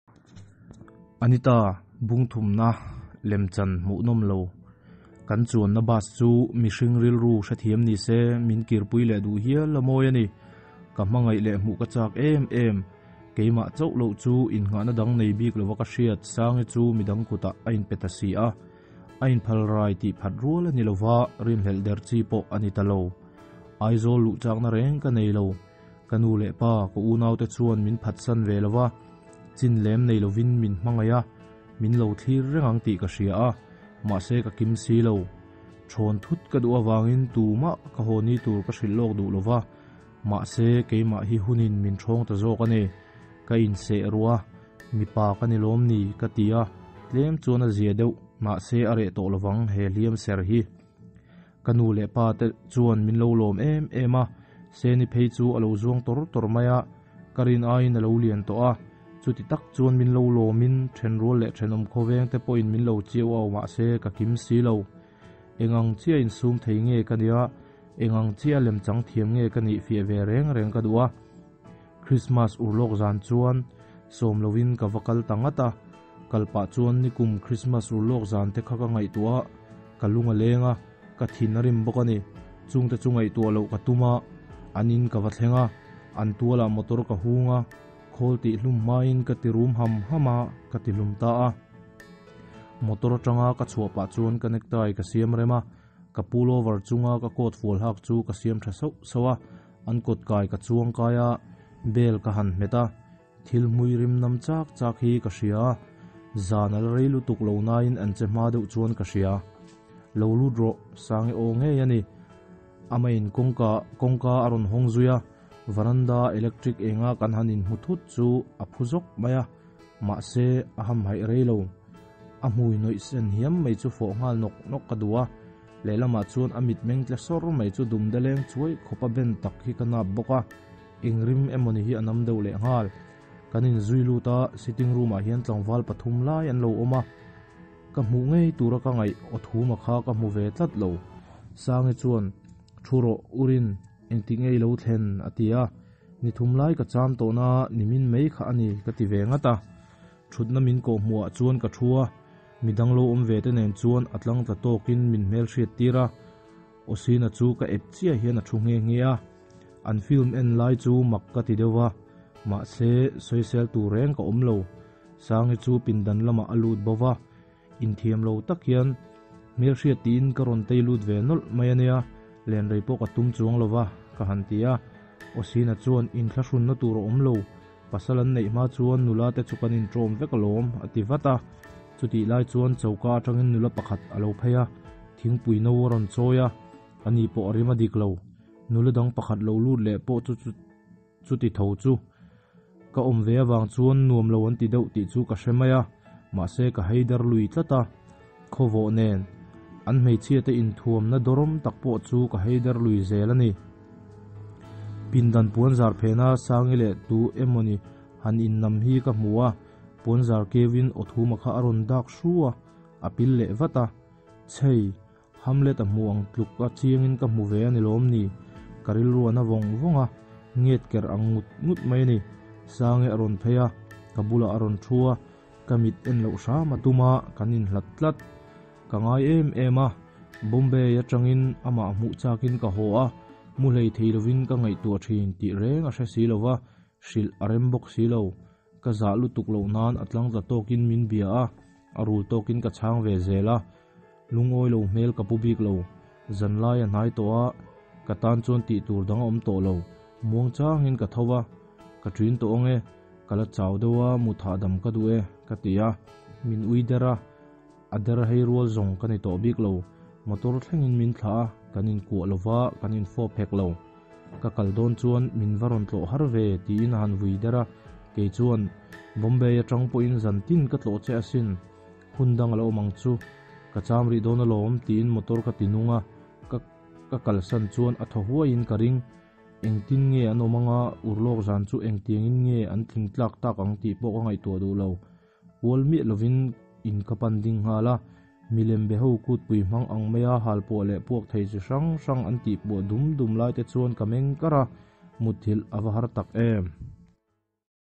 Mizo Thawnthu ( Anita - C.Laizawna) Bung - 3 na
He Thawnthu hi a ziaktu Pu C.Laizawna remtihna thlapa dah luh a ni e.Tin, he thawnthu ngaihnawm tak hi kum 1998 ah khan MIZO Academy of Letters Book of the Year ah thlan a ni tawh nghe nghe.